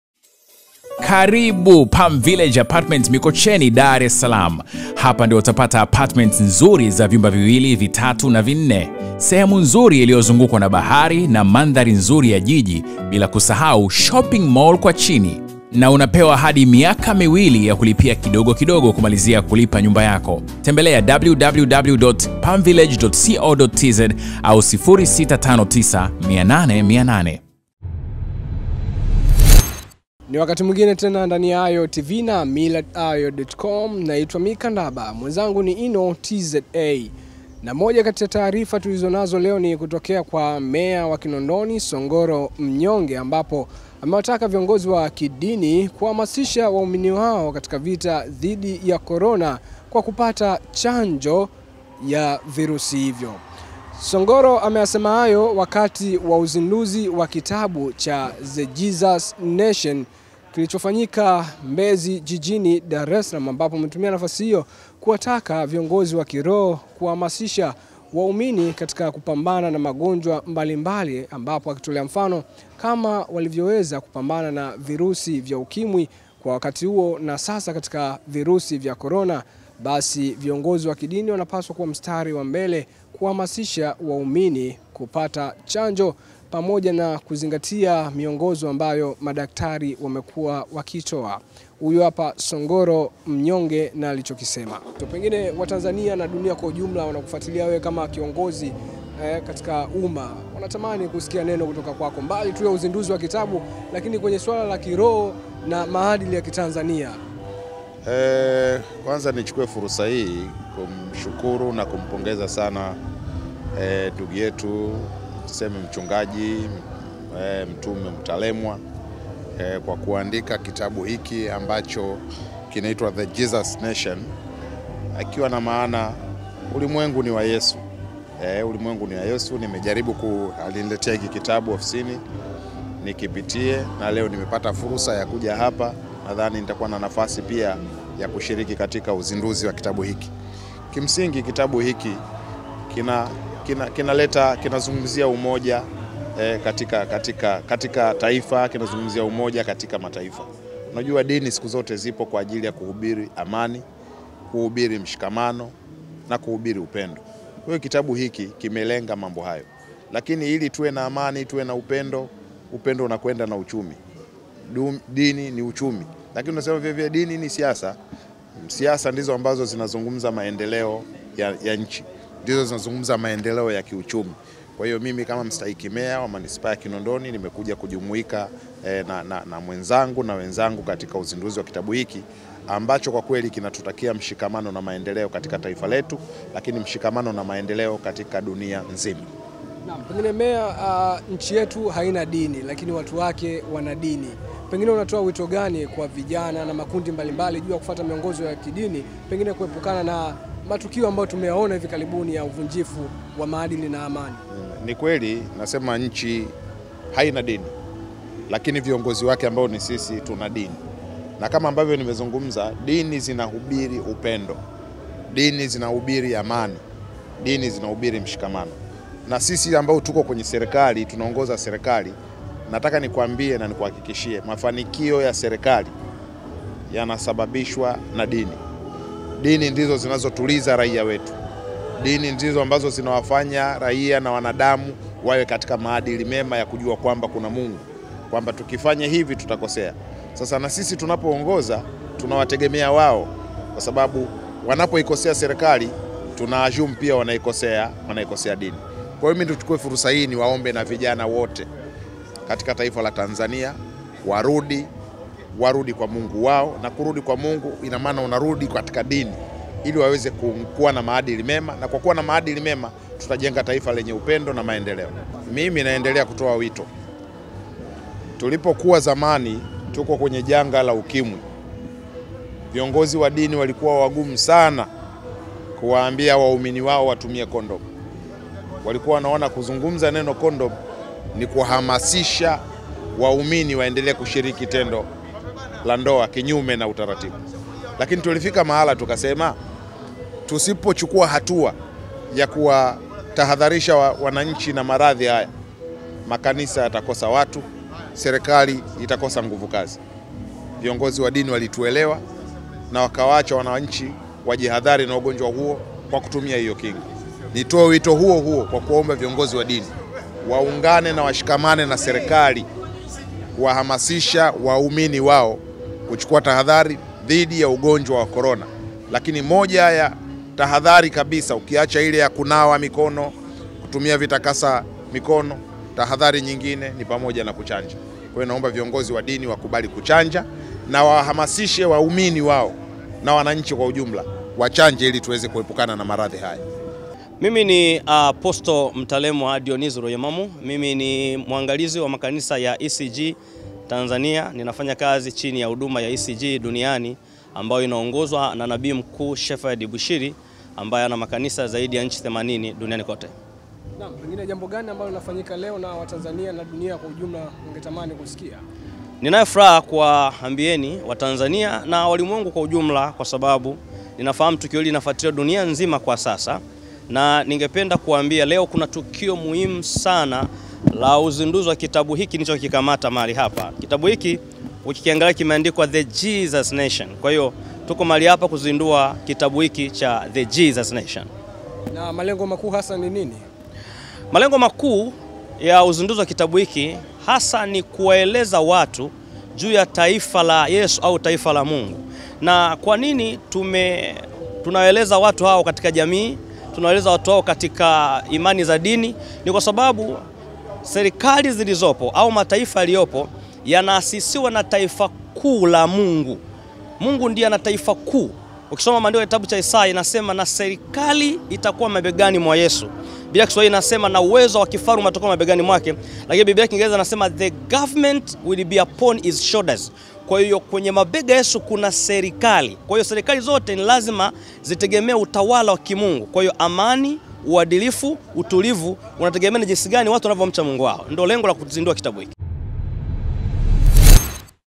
CHANJO YA CORONA YAMUIBUA MEYA KINONDONI "WALIKUWA WAGUMU, VIONGOZI WA DINI WACHANJE"
Meya wa Kinondoni, Songoro Mnyonge amewataka viongozi wa Dini kuwahamasisha waumini wao kupambana na virusi vya COVID-19 kwa kuchukua hatua zote ikiwemo kuchanjwa.